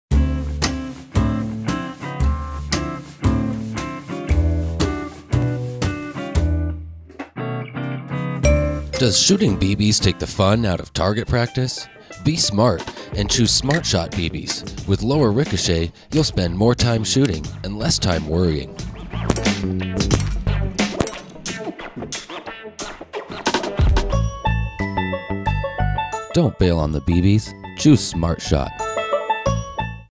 Air Venturi H&N Smart Shot BBs
Steel BBs ricochet. They ricochet BAD! Better NOT be shooting at any hard surfaces. That is unless you are using copper-coated Smart Shot Lead BBs - low ricochet and you can enjoy shooting targets you could not before!